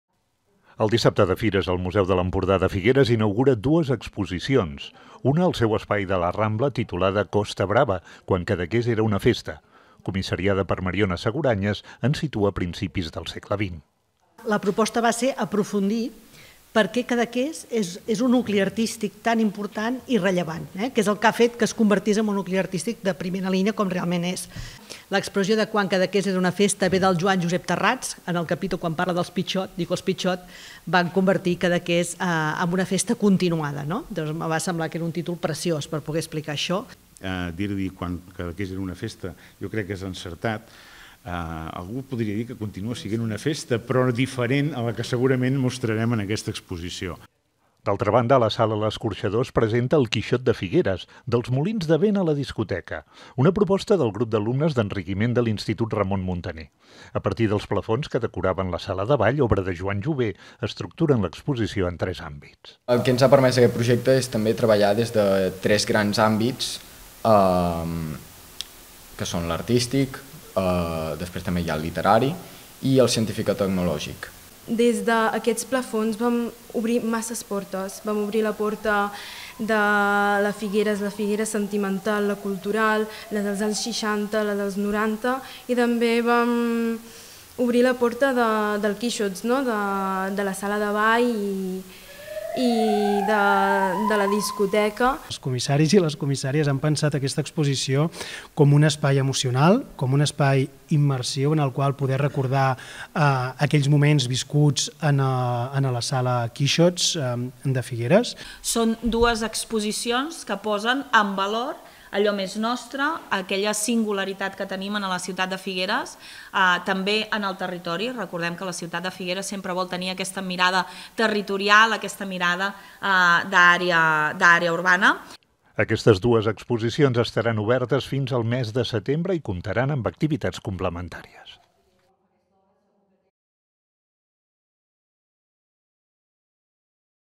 Dues exposicions de Fires del Museu de l'Empordà per a fer memòria
El Museu de l'Empordà inaugurarà dues exposicions per Fires. A la seva seu de la Rambla "Costa Brava, quan Cadaqués era una festa" i a la Sala l'Escorxador, "El Quixot de Figueres, dels molins de vent a la discoteca".